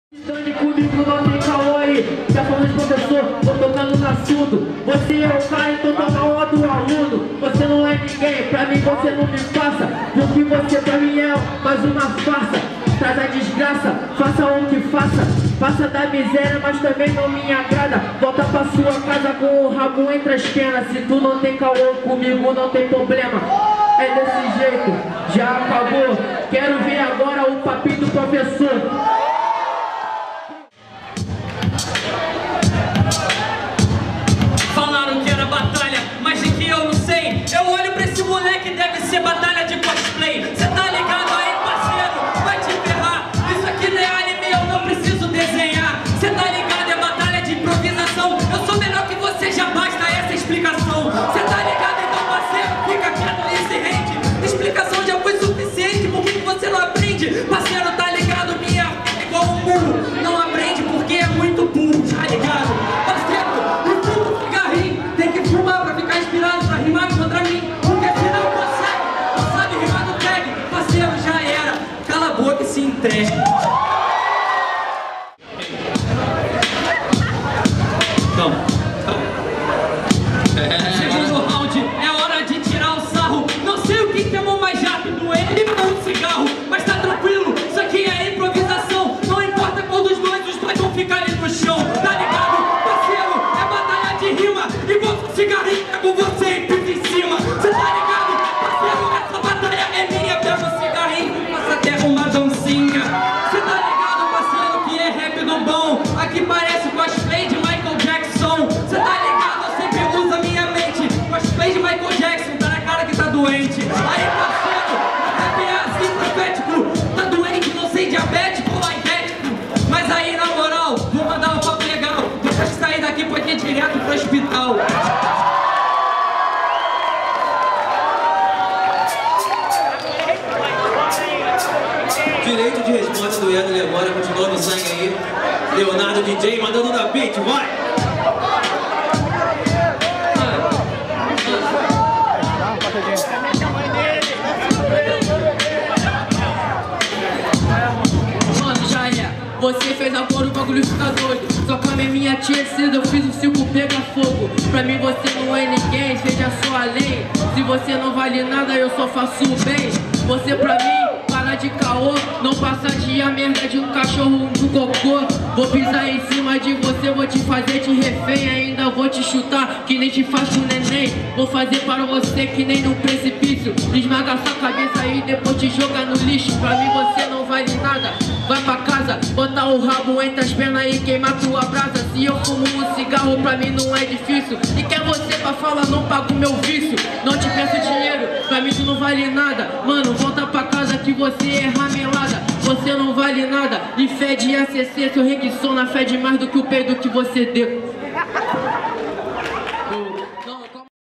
MC Sergio vs Edhy - Final da 6º Edição da Batalha da Rodô - Cachoeira de Macacu -
6º Edição da Batalha da Rodô em Cachoeira de Macacu - RJ Vídeo da Final que consagrou o mc Sergio como o grande campeão dessa edição.